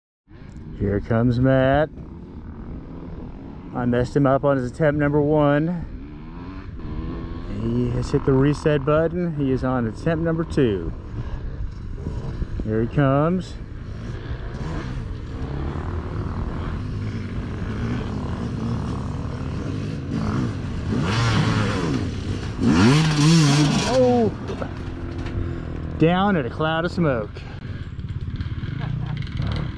YZ250X Matt Fail
Matt makes run number two after I caused him to fail hill clime attempt number one.